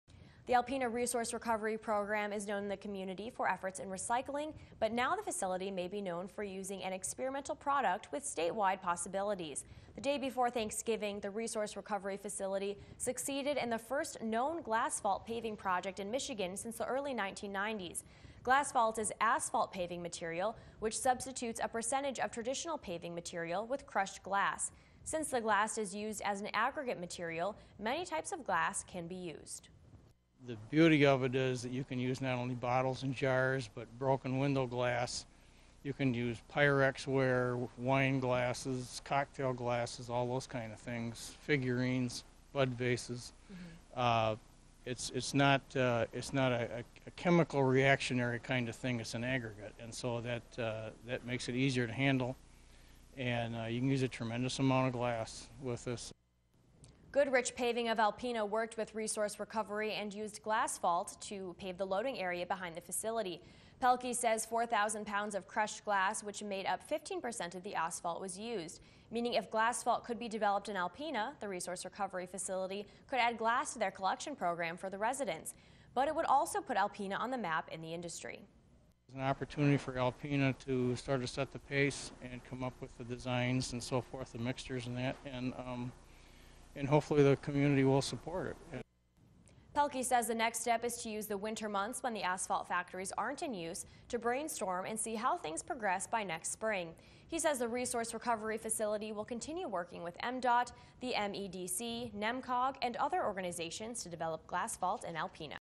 Resource Recovery Center's "Glassphalt"
Alpena's Resource Recovery Facility has paved a portion of road behind their facility with glassphalt. Read here for more information on this new project.